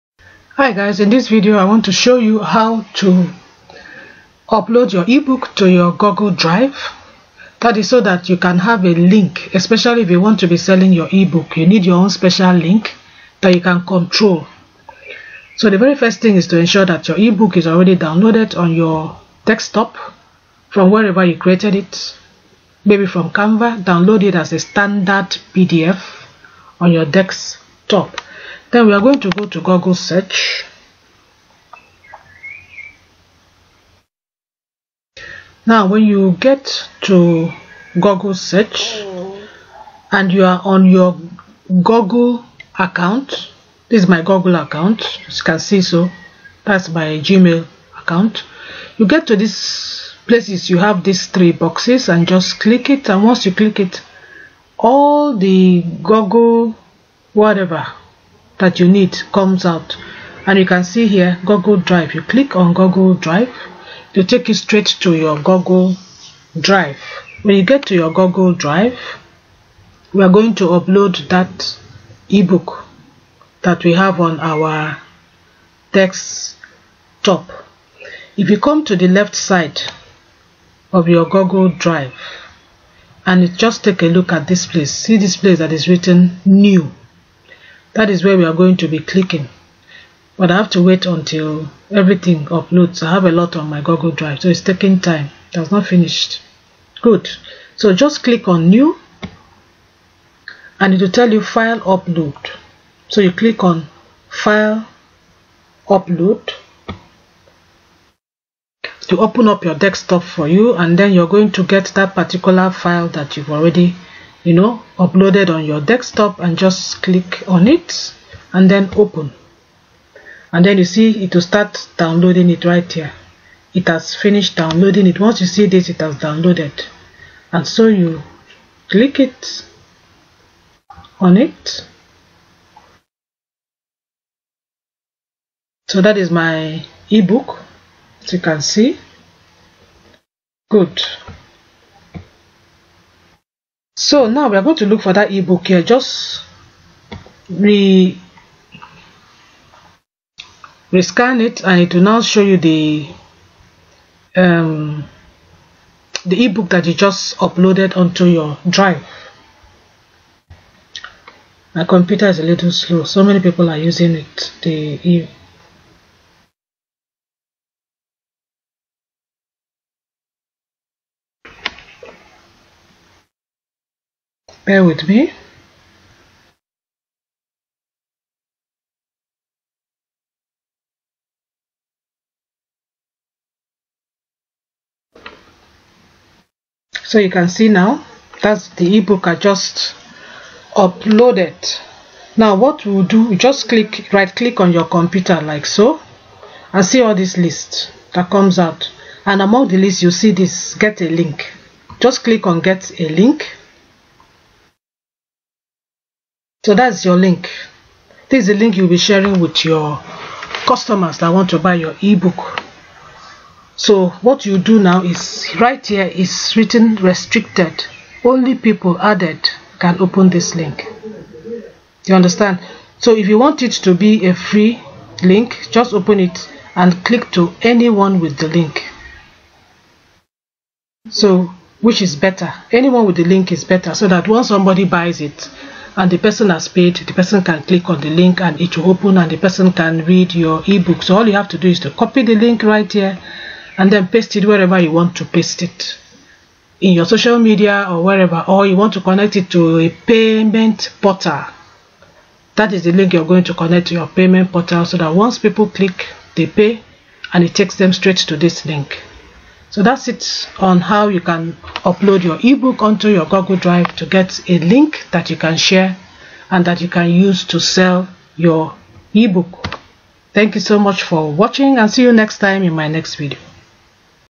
HOW TO GET AND SHARE AN EBOOK LINK USING GOOGLE DRIVE/how to sell your e books as a digital product
This is a short video on how to get and share an ebook link using google drive.this is a Beginner friendly method. This is particularly useful when you want to sell your ebooks as digitalproducts online.